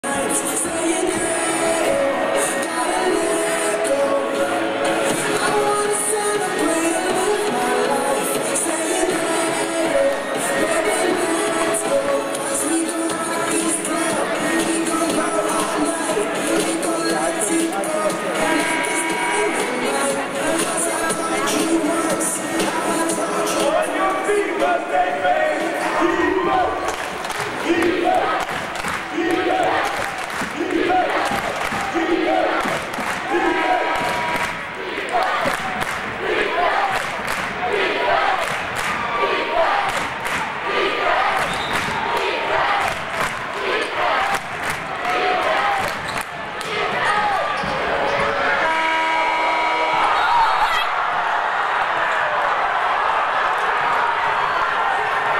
BSU Dermaine Crockrell's last second game winner at Southwest Minnesota State (Feb. 17, 2012)
With 2.3 seconds showing on the clock and down two, junior Dermaine Crockrell (G, Mesa, Ariz.) got the inbound pass from the opposite baseline and knocked down a desperation shot from just inside half court to lift Bemidji State University men's basketball team to a 79-78 victory at Southwest Minnesota State University Friday.